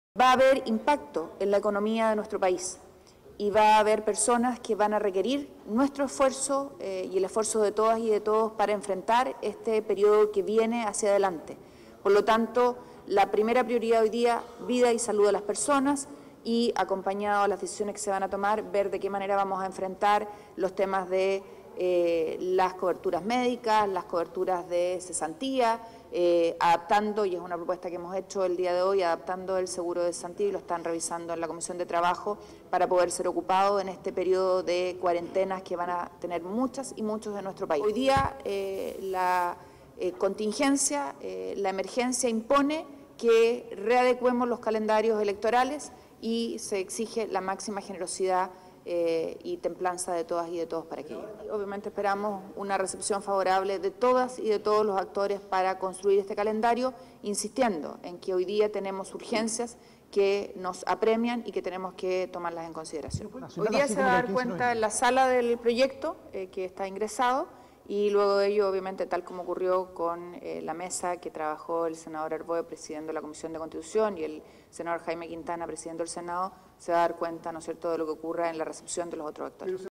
Hoy presenté proyecto de reforma constitucional para posponer el plebiscito sobre nueva Constitución
Hoy presenté un proyecto de reforma constitucional para posponer el plebiscito sobre nueva Constitución hasta el 06 de septiembre. Escucha mis fundamentos para esta decisión en este vídeo